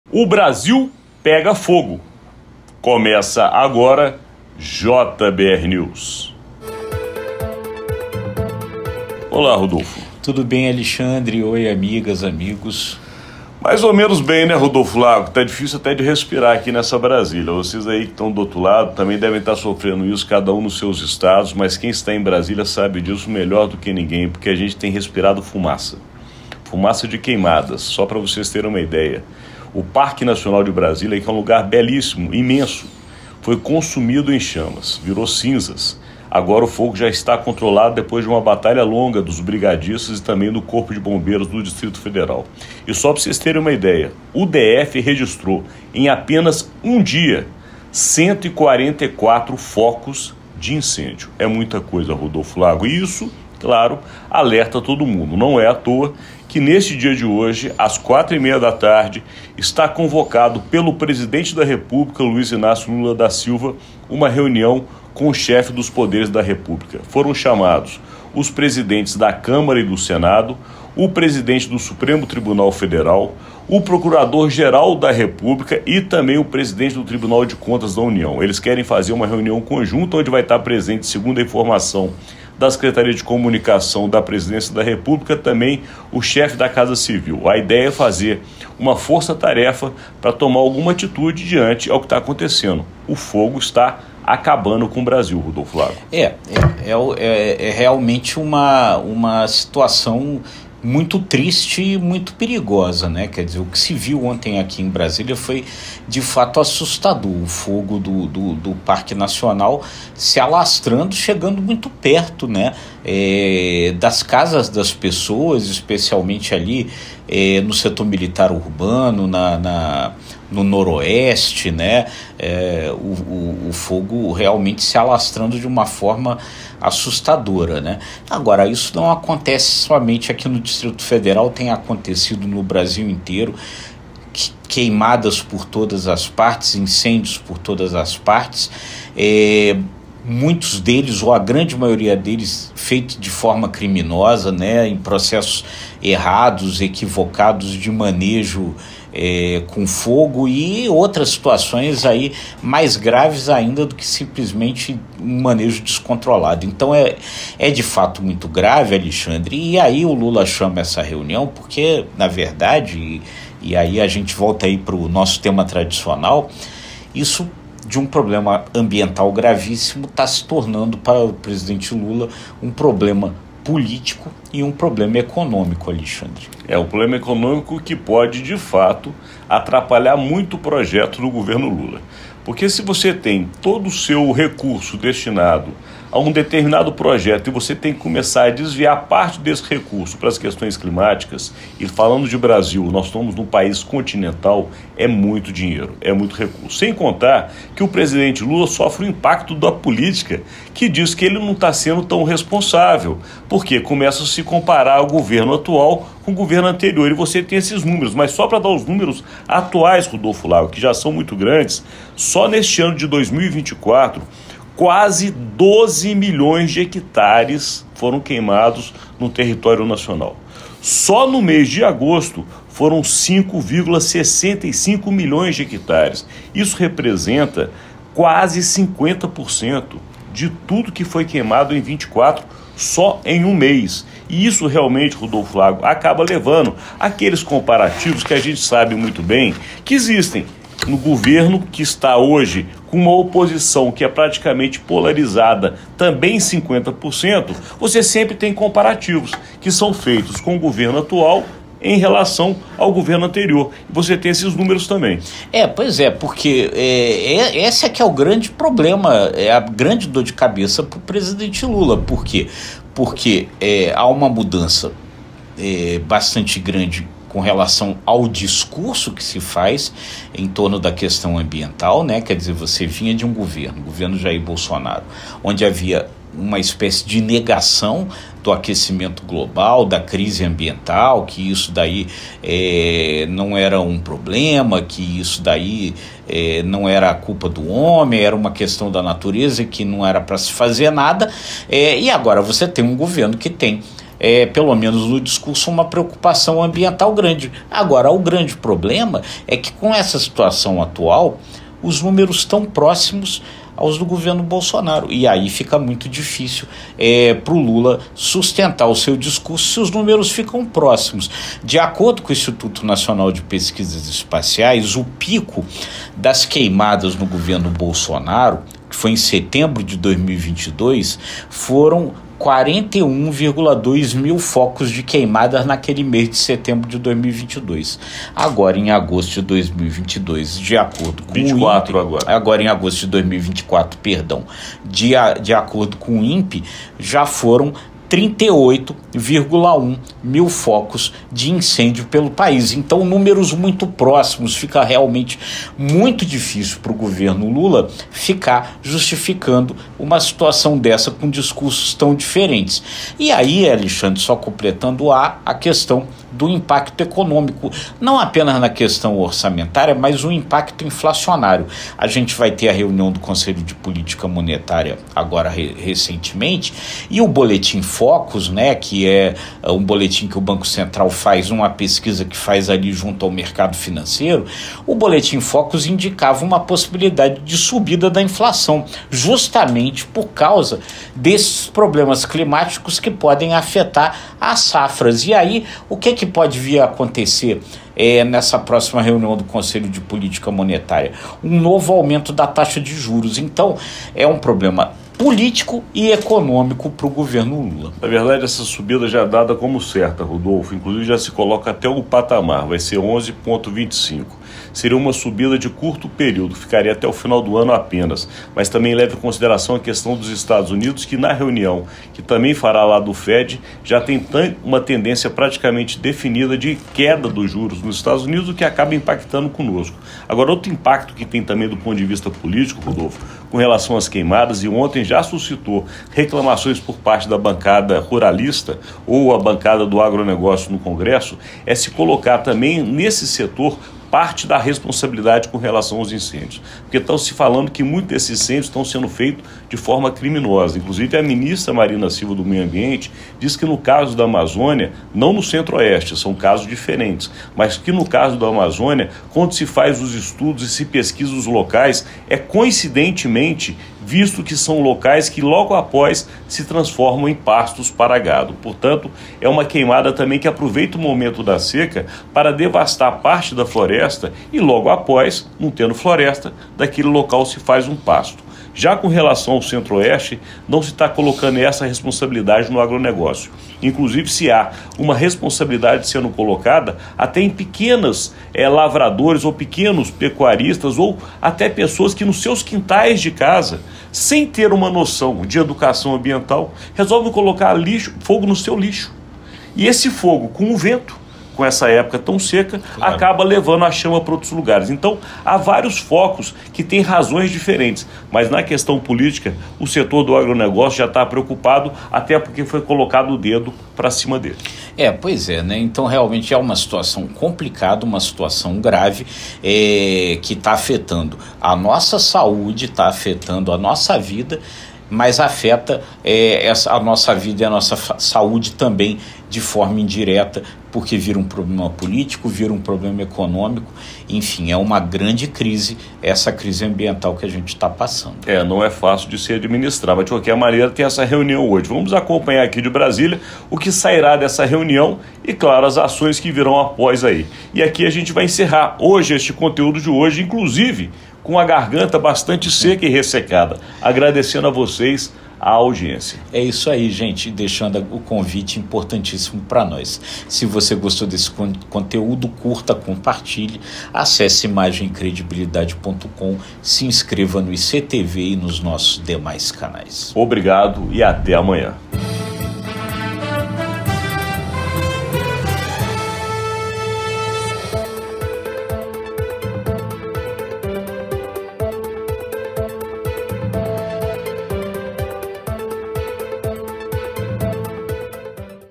JBr News - O Brasil queima! Dor de cabeça para Lula
O presidente Luiz Inácio Lula da Silva convocou para a tarde desta terça-feira (17) uma reunião com os chefes dos demais poderes para tratar da grave crise ambiental que assola o país. Brasília viveu na segunda-feira (16) um dia assustador, com incêndios por toda a volta, chegando muito próximos das casas das pessoas, especialmente no Setor Militar Urbano e no Noroeste. Mas não é somente em Brasília o problema: as queimadas se espalham por todo o país. E acabam virando um problema político e econômico para o governo Lula. Econômico, porque os incêndios afetam as safras, e isso vai impactar no preço dos alimentos, e pode mesmo levar o Comitê de Política Monetária (Copom) a elevar novamente a taxa de juros. Político, porque o governo Lula tem um discurso de preservação do meio ambiente, e os picos das queimadas no país já estão atingindo números muito próximos dos picos no governo Jair Bolsonaro, que não tinha essa preocupação. É o que mostram Alexandre Jardim e Rudolfo Lago no JBrNews de hoje.